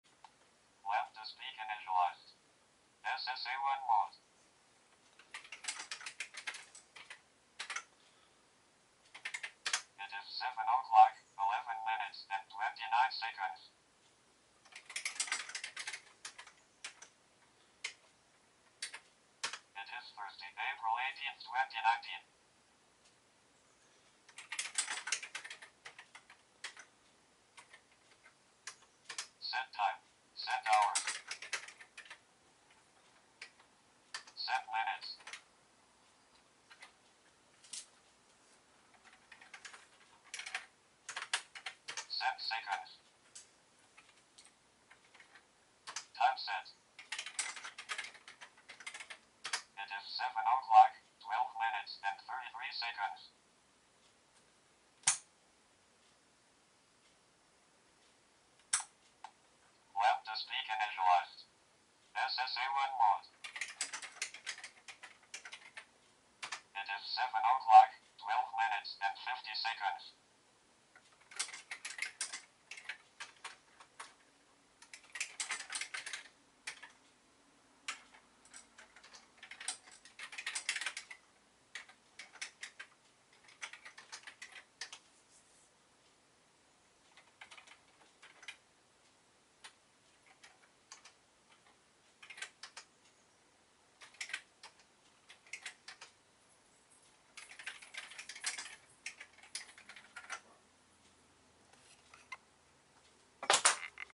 LambdaSpeak 3 - Real Time Clock, Calendar, and Temperature Sensor Test
LambdaSpeak 3 got an RTC option - the 4 $ DS3231 I2C module is supported. This adds a battery buffered clock, calendar, and a temperature sensor to LambdaSpeak!

Currently, the clock module is just glued to the back of LS 3 and connected via some hand-soldered wires. The final PCB will have a proper mounting header for soldering in the clock module.

The RTC module uses I2C for communication.

One can get a 5 pack of these for arround 15 $ on Ebay. Due to the temperature sensor for clock drift compensation, this is a high precession clock (better than DS1307).